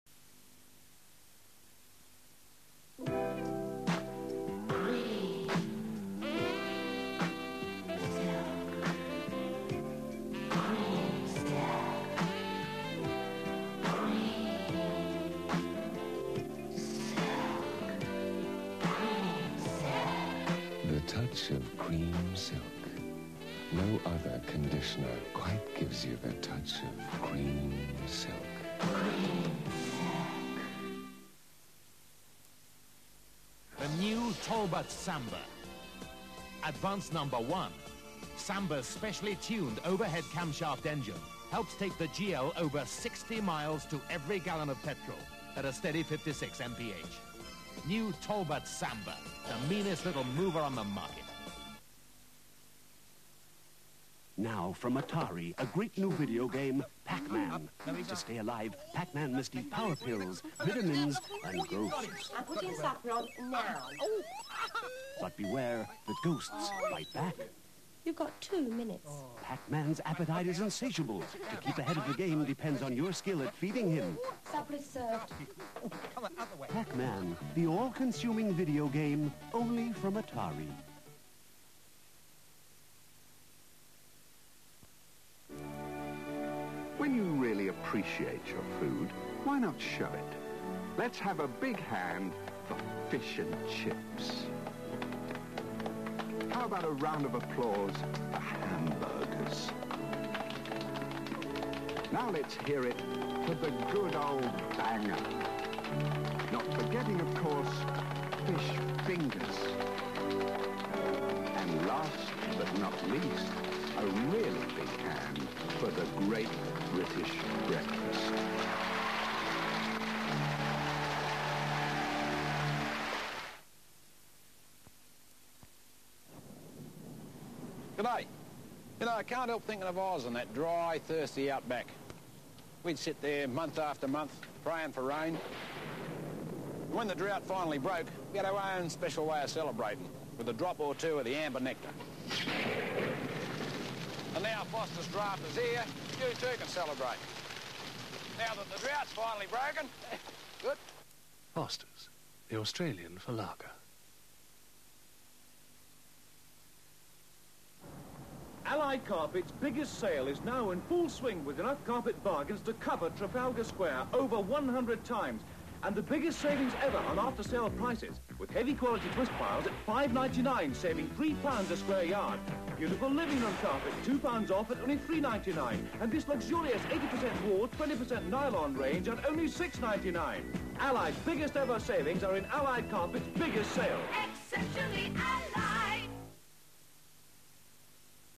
TVS commercial break (June 1982)
Channel: ITV (TVS)
TX: 14th June 1982

Cream Silk Shampoo
Talbot Samba
Atari VCS & Pac-Man
Heinz Ketchup
Fosters Lager
Allied Carpets